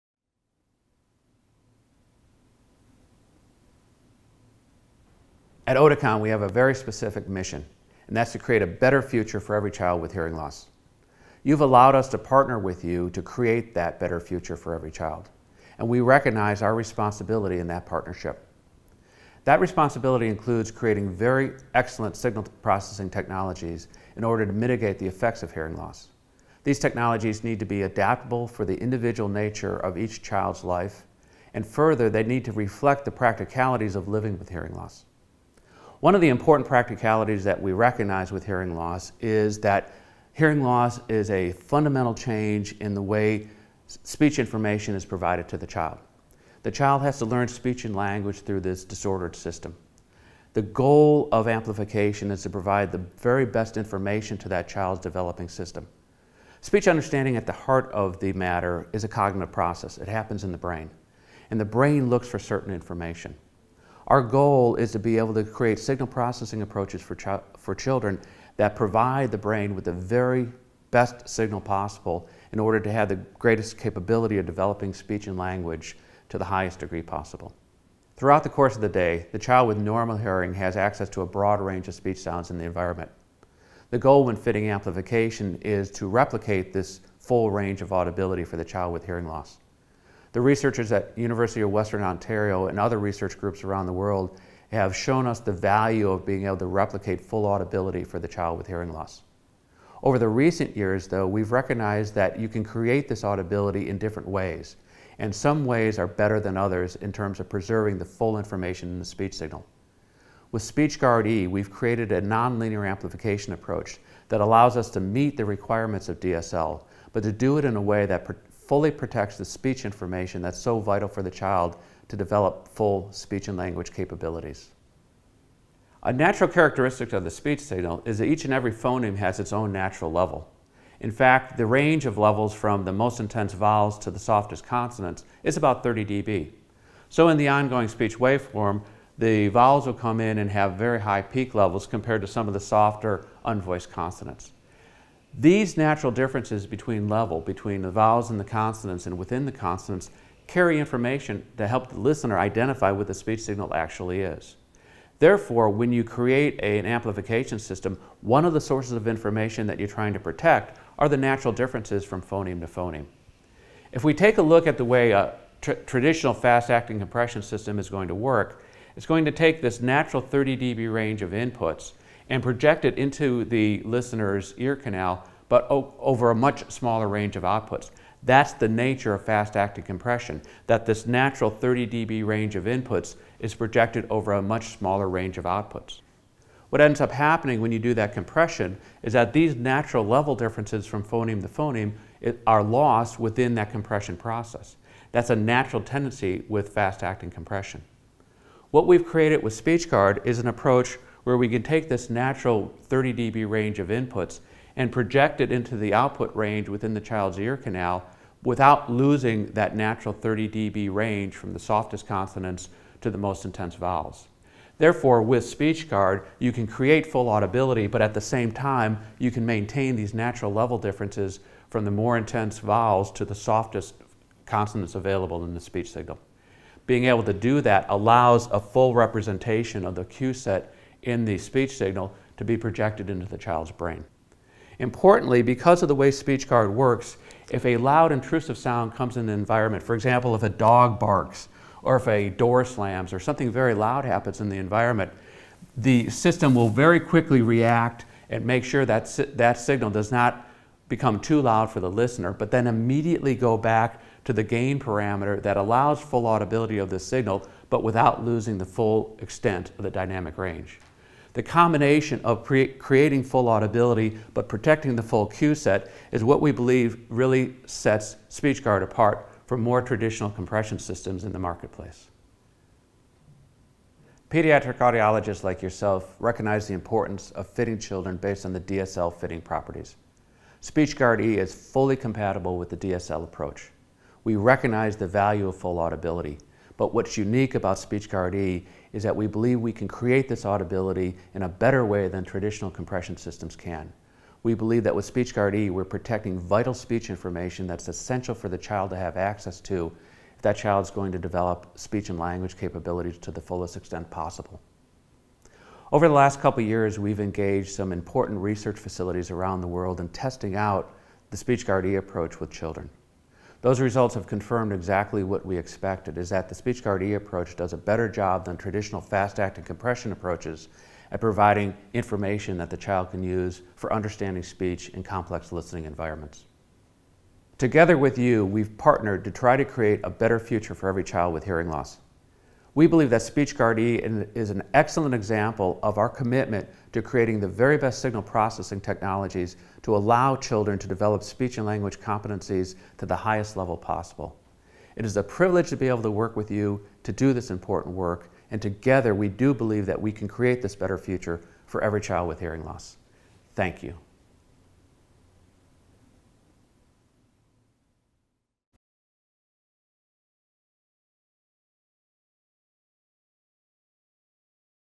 Oticon Sensei Podcast - Speech Guard E
Dr. Donald Schum talks about the audiology feature Speech Guard E. Speech Guard E is a part of the dedicated Paediatric hearing instrument, Oticon Sensei.

More about Donald J. Schum (PhD in audiologist):
After graduating from the University of Illinois in 1982 with a bachelor's degree in speech and hearing science, Dr. Donald Schum went on to earn a master's degree in audiology at the University of Iowa in 1984 and a doctoral degree in audiology at Louisiana State University in 1988. Throughout his professional career, he has been engaged in research on hearing aids, speech understanding, and aging.